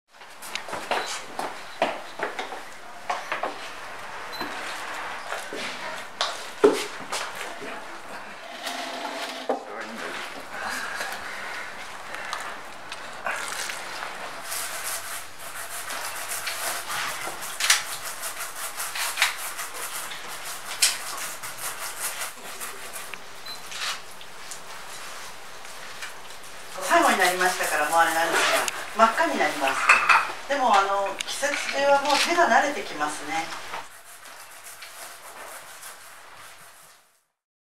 新米できりたんぽづくり 料亭「濱乃家」 秋田
秋田市大町４丁目の料亭「濱乃家（はまのや）」で９月２３日、地元産の新米を使ったきりたんぽづくりが始まった。